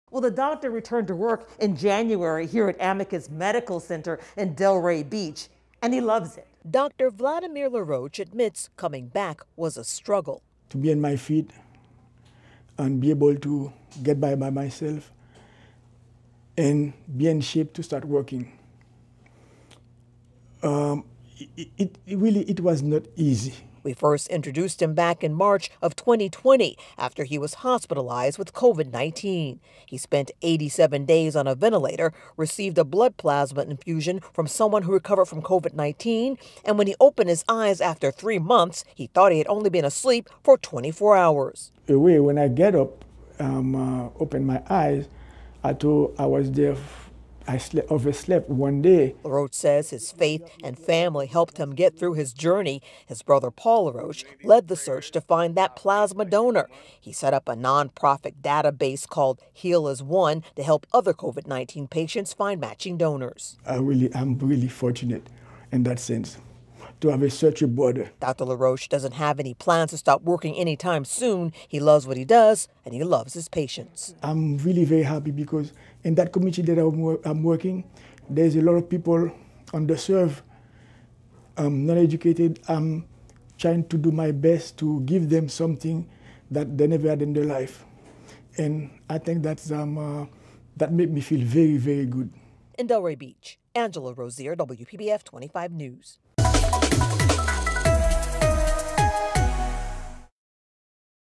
Local doctor returns to work after recovering from COVID-19
WPBF's Angela Rozier has the story.